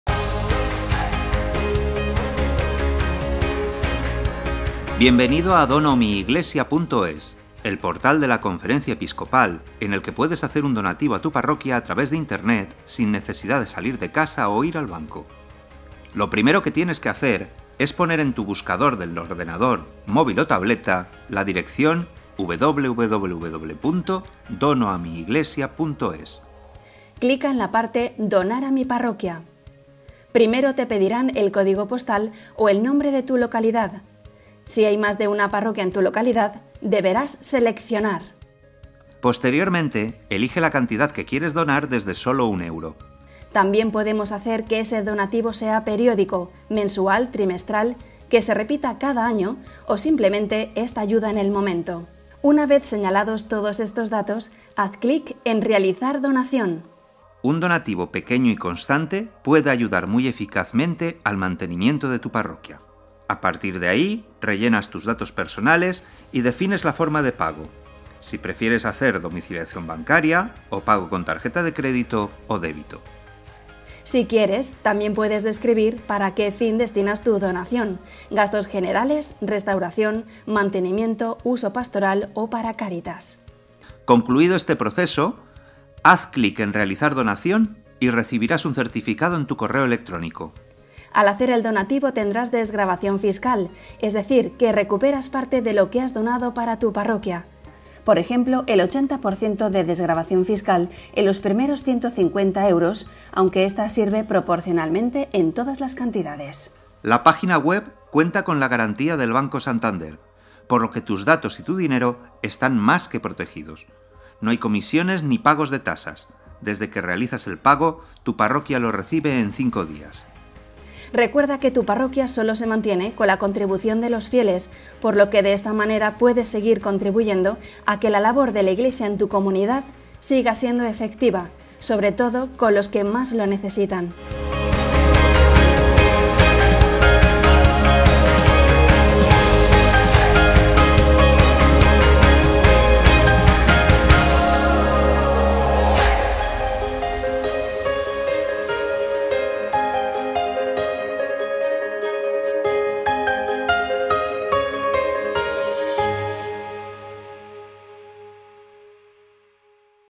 Ayudar a tu parroquia es muy fácil
Así puedes ayudar a tu parroquia siguiendo los sencillos pasos de este tutorial.
En la web donoamiiglesia.es, puedes donar a tu parroquia, a la diócesis o a la Conferencia Episcopal Española. Puedes elegir entre hacer una donación puntual, mensual o anual.
Además, por cada donación menor de 150 euros tienes una desgravación fiscal del 80%, mientras que lo que dones a partir de esa cantidad desgrava al 35%.
Ayuda a tu parroquia. Ayúdate.
Es tiempo de aumentar la CARIDAD cuando todo está cerrado, de abrir puertas para ACOGER, de estar cerca de QUIEN LO NECESITA
Somos parte de una comunidad, de una IGLESIA comprometida y sin miedo.
¿Y TÚ, ya eres parte?